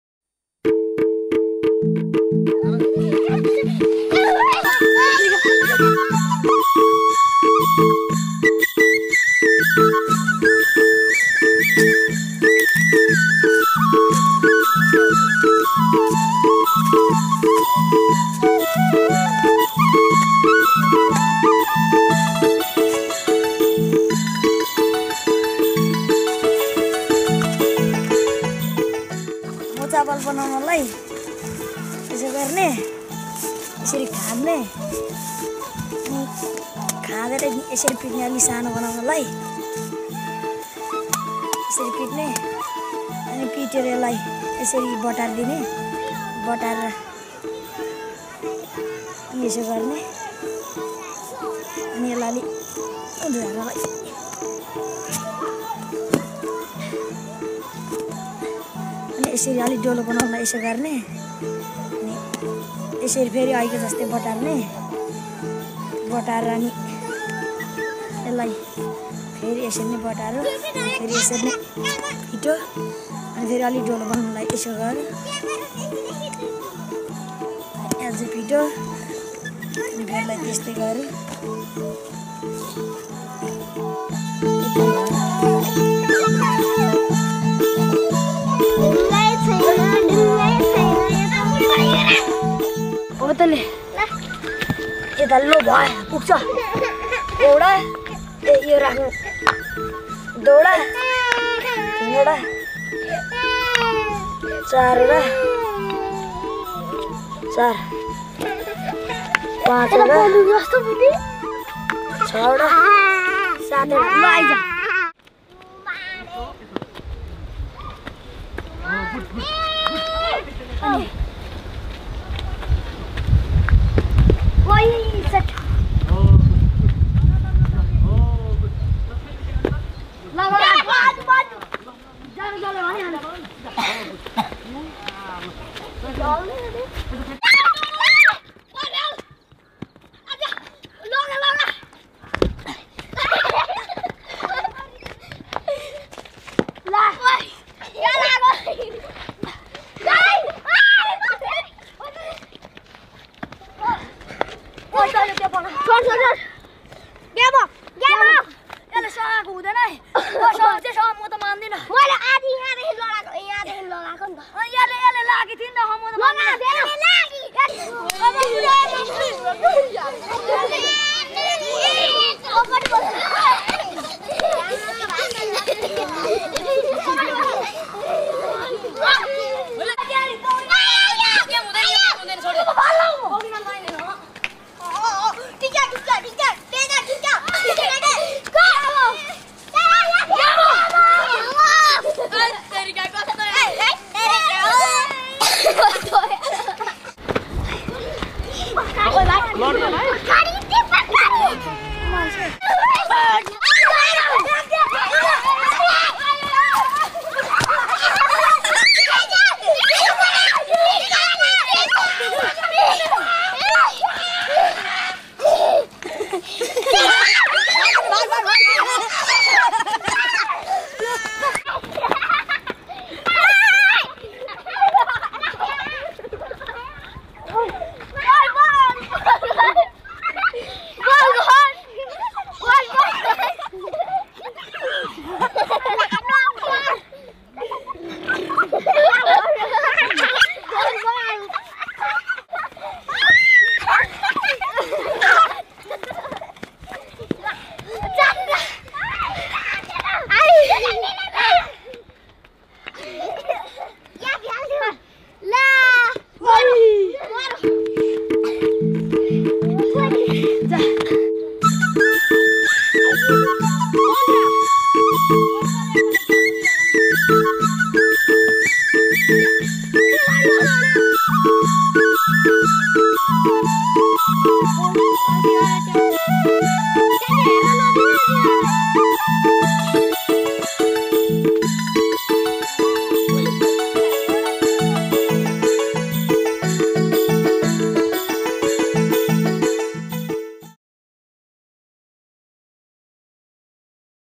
Seven Stone
This video is about a traditional and very popular game in Nepal